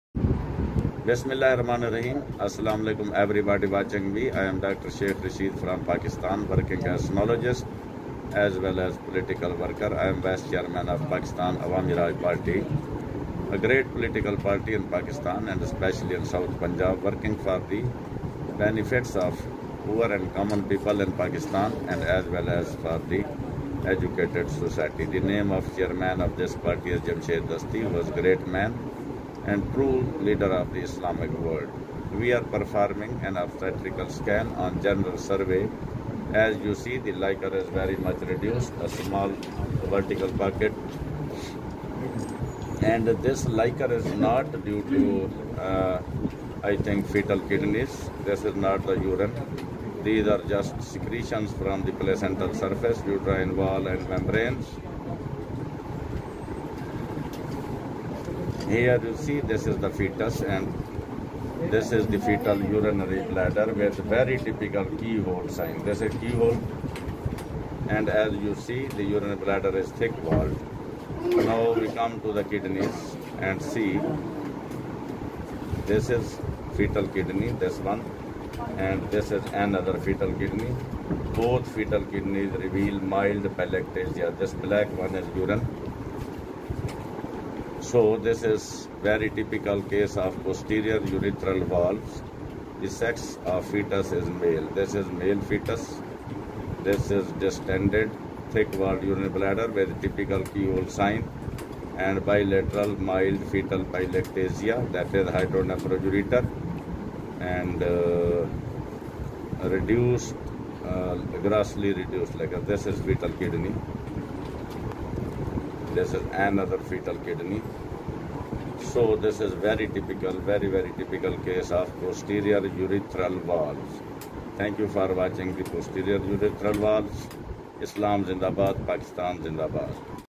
Posterior Urethral Walls
Posterior Urethral Walls ultrasound video by Dr,Abdul rasheed sheikh  Ultrasound Speasilist in Pakistan